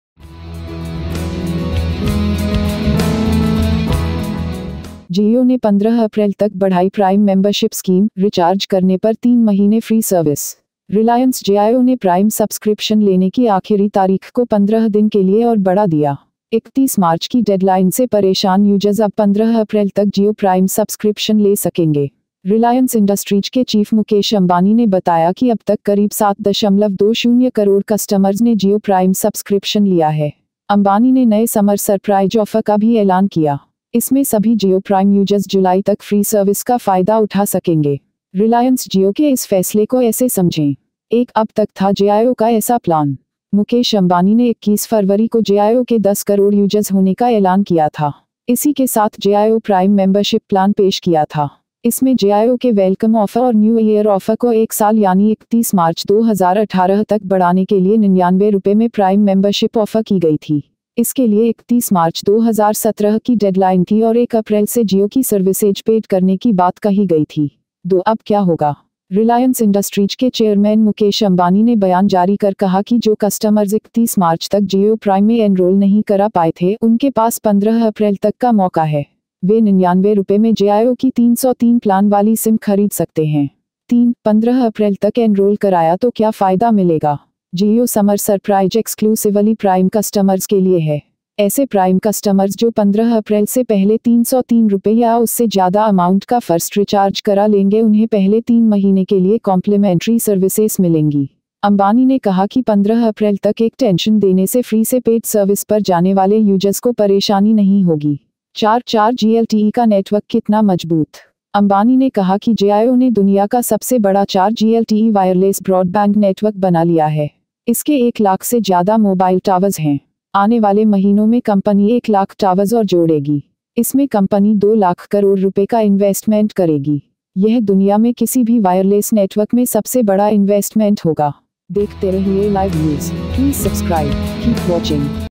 Jio ने 15 अप्रैल तक बढ़ाई प्राइम मेंबरशिप स्कीम, रिचार्ज करने पर 3 महीने फ्री सर्विस
Jio extended up to 15th April 2017 Prime Membership scheme, 3 months free service on recharging

Reliance Jio ने Prime प्राइम subscription सब्सक्रिप्शन लेने की आखिरी तारीख को 15 दिन के लिए और बढ़ा दिया। 31 मार्च की डेडलाइन से परेशान यूजर्स अब 15 अप्रैल तक जियो प्राइम सब्सक्रिप्शन ले सकेंगे। रिलायंस इंडस्ट्रीज के चीफ मुकेश अंबानी ने बताया कि अब तक करीब 7.20 करोड़ कस्टमर्स ने जियो प्राइम सब्सक्रिप्शन लिया है। इसमें सभी Jio Prime यूजर्स जुलाई तक फ्री सर्विस का फायदा उठा सकेंगे। रिलायंस जियो के इस फैसले को ऐसे समझें...